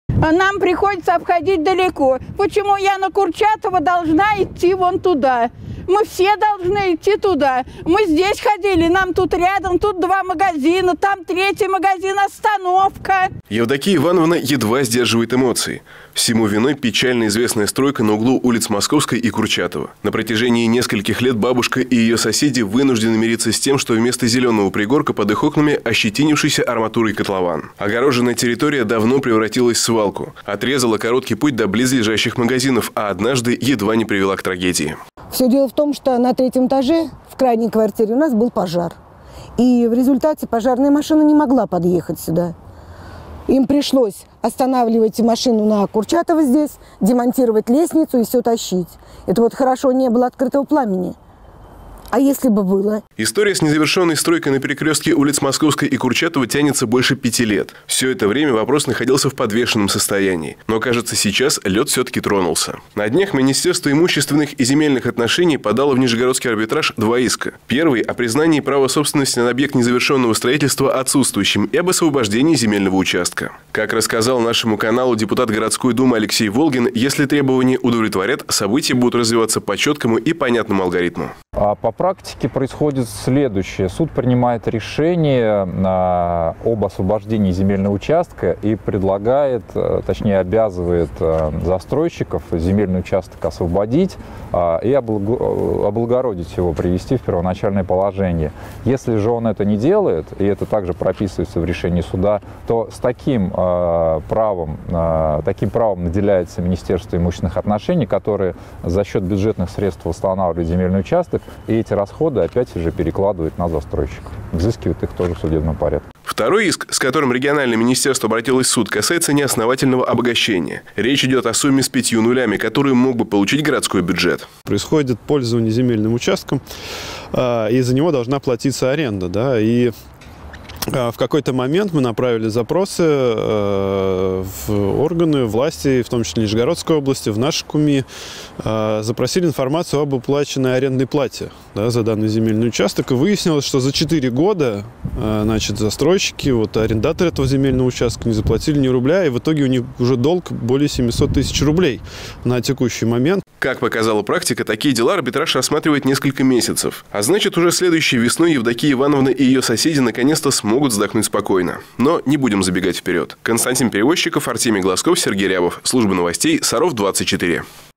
Что будет с долгостроем на ул. Курчатова?
Долгострой на перекрестке улиц Московской и Курчатова на протяжении пяти лет остаётся актуальной проблемой для жителей близлежащих домов и всего города. Но дело сдвинулось с мёртвой точки. Министерство имущественных и земельных отношений подало в нижегородский арбитраж два иска.